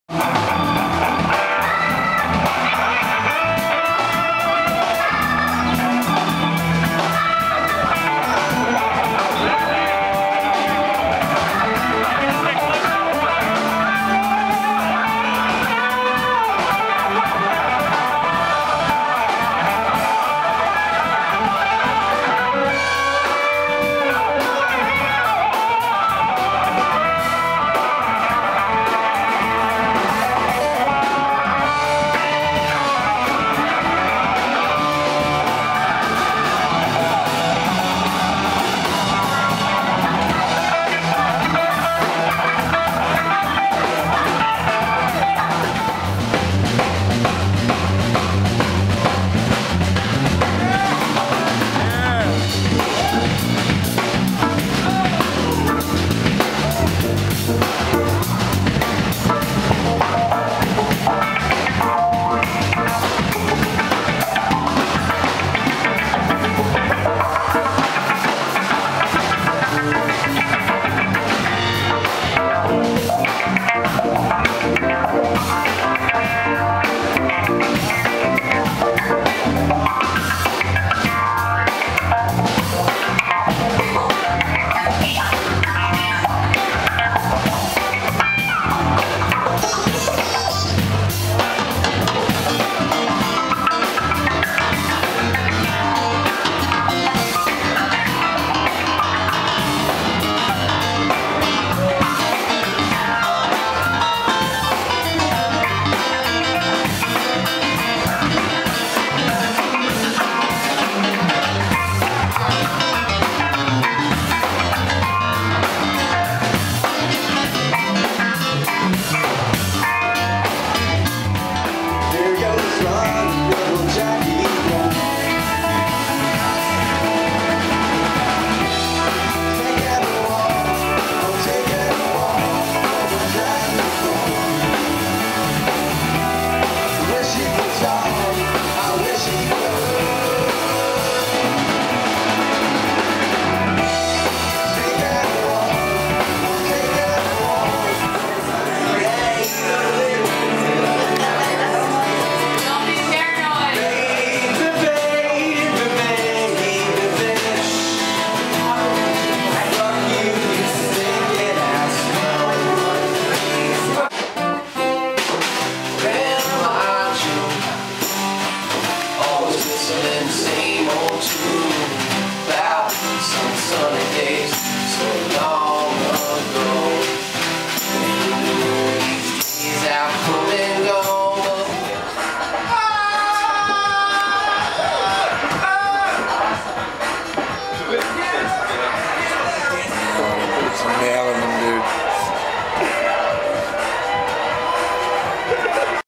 The Heavy Pets
Excerpts from an AWESOME night with the Heavy Pets.